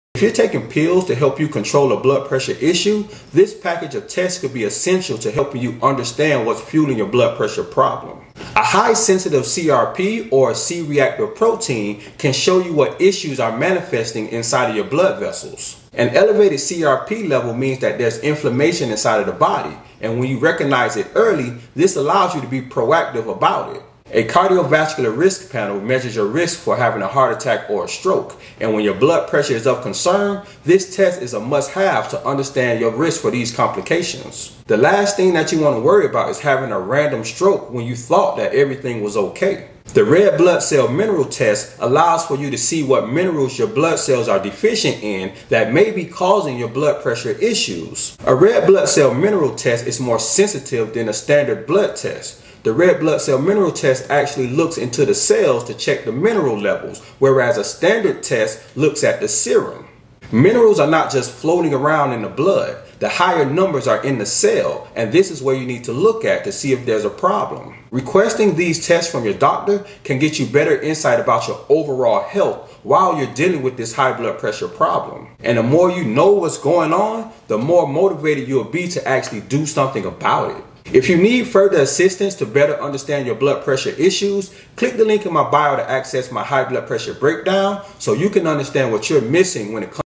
The Diagnostic Trio: How These Tests Can Transform Your Blood Pressure Management
Many times doctors default right to the prescription pad instead of taking a deeper look to search for the root cause of the problem.

Test 1- hsC-reactive Protein
Test 2- Cardiovascular Risk Panel
Test 3- RBC Mineral Test